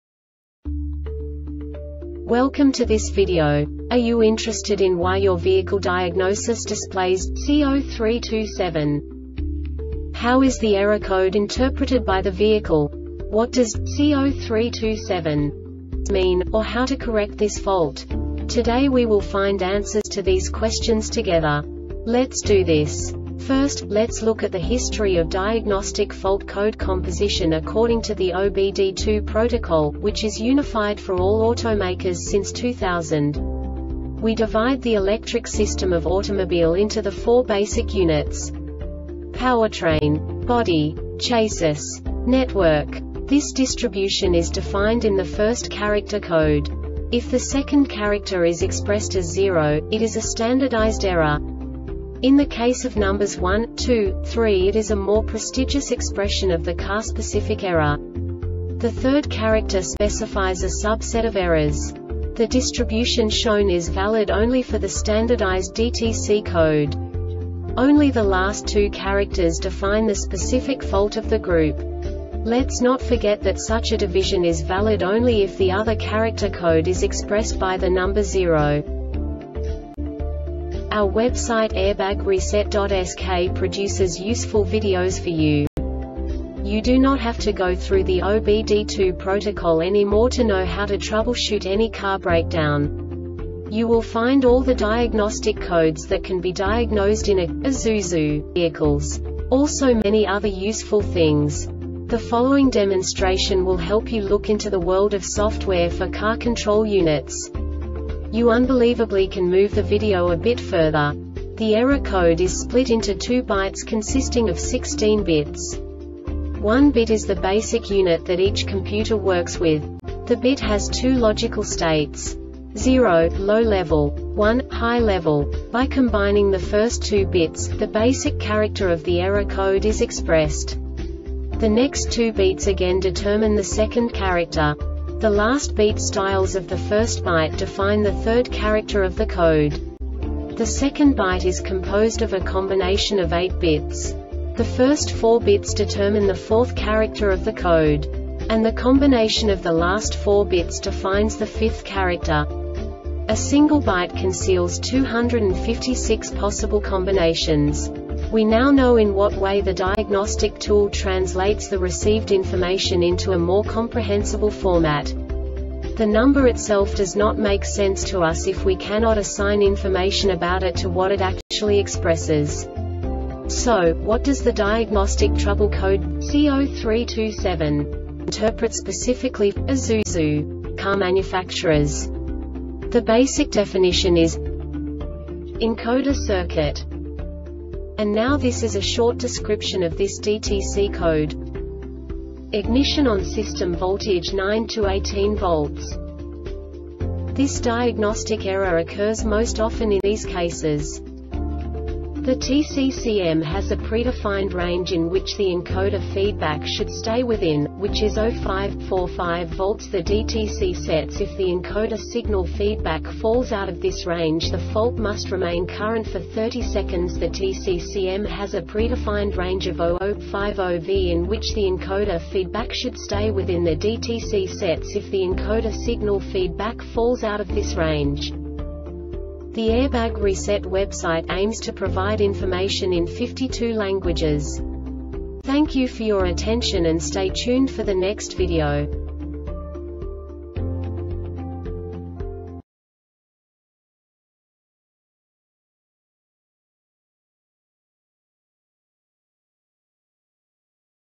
DTC Isuzu C0327 Short Explanation
Contents:
0:21 Basic DTC analysis according to OBD2 protocol standard.
1:48 Insight into programming
2:58 A diagnostically understandable description of the Diagnostic Trouble Code
3:05 Basic error definition

Make:
Isuzu
Code:
C0327
Definition:
Encoder Circuit
Description:
Ignition ON.System voltage 9-18 V.
Cause:
The TCCM has a predefined range in which the encoder feedback should stay within,which is 0.5-4.5 volts.The DTC sets if the encoder signal feedback falls out of this range.The fault must remain current for 30 seconds.The TCCM has a predefined range of 0.0-5.0 V in which the encoder feed back should stay within.The DTC sets if the encoder signal feed back falls out of this range.